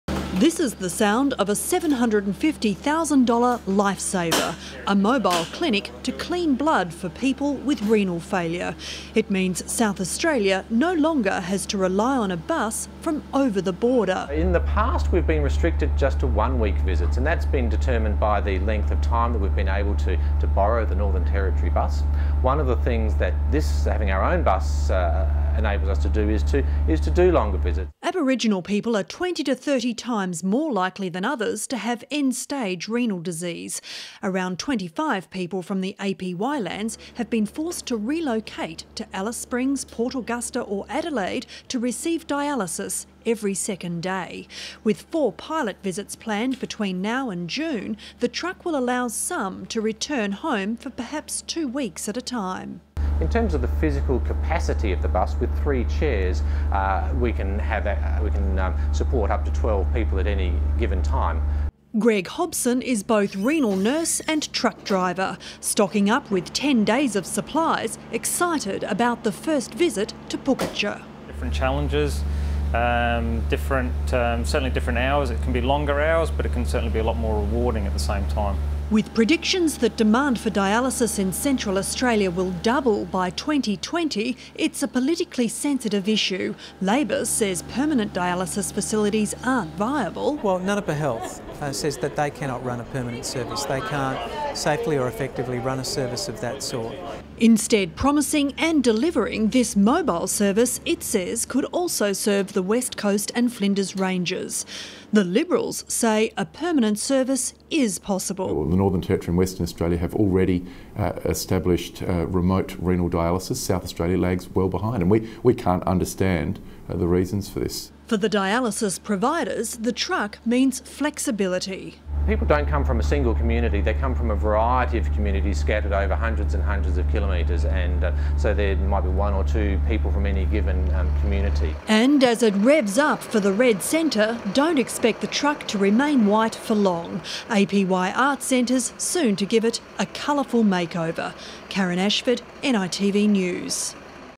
Mobile dialysis truck comes to SA's APY lands
For the first time, a regular renal dialysis service is about to become available on South Australia's APY lands.
A mobile dialysis truck will enable people with kidney disease to receive treatment in their home communities.